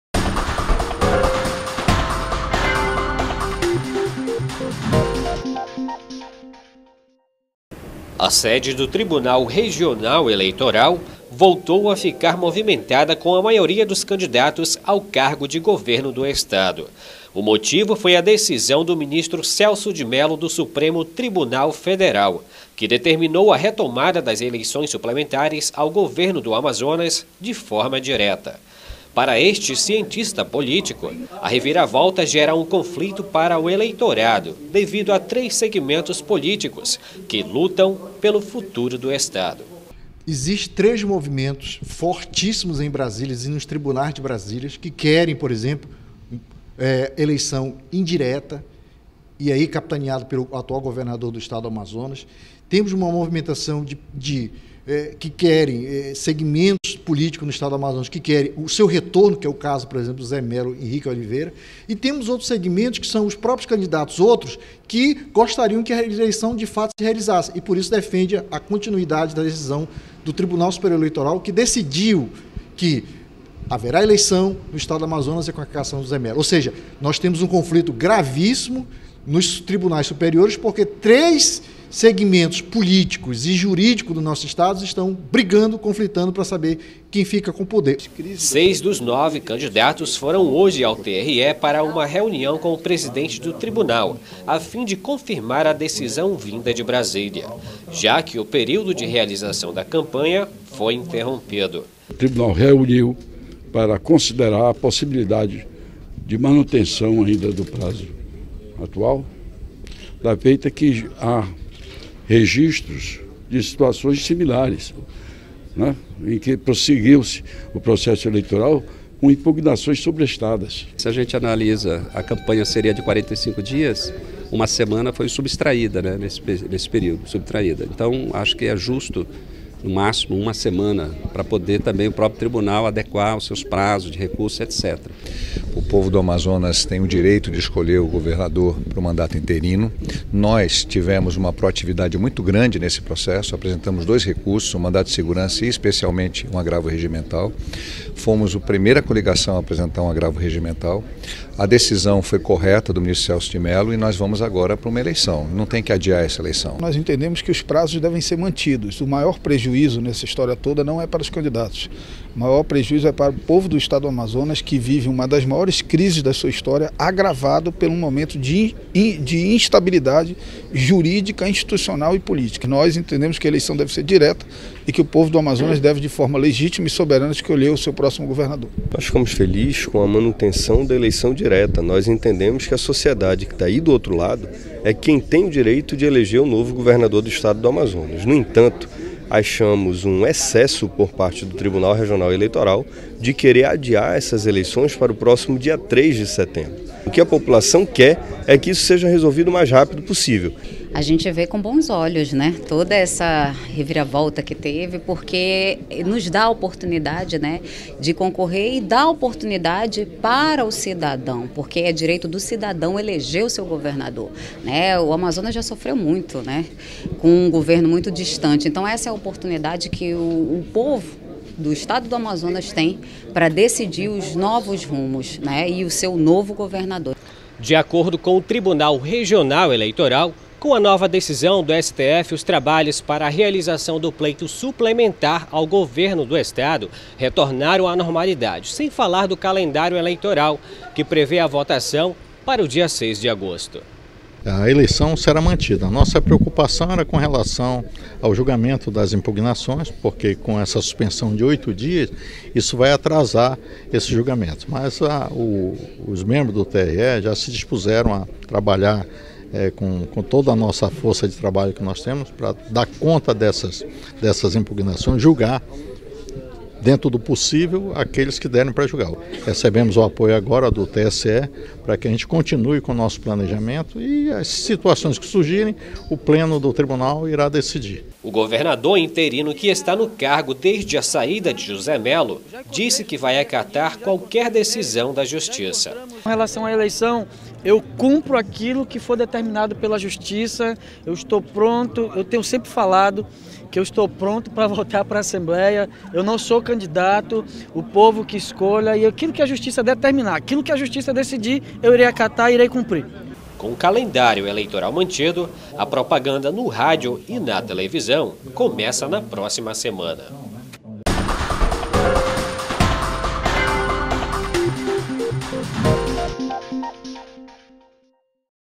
Após decisão no STF, campanha ao governo do Amazonas está de volta
Os rumos da eleição suplementar no Amazonas mudaram de direção novamente. E o Tribunal Regional Eleitoral (TRE) já trabalha para realizar a votação que vai definir o novo governador do estado no dia 6 de agosto. A mudança ocorreu por conta de nova decisão do STF, do ministro Celso de Mello, que entendeu que o recurso que foi deferido liminarmente pelo ministro Ricardo Lewandowski no dia 28 de junho deveria ter sido enviado para o Tribunal Superior Eleitoral (TSE), e não para a instância superior.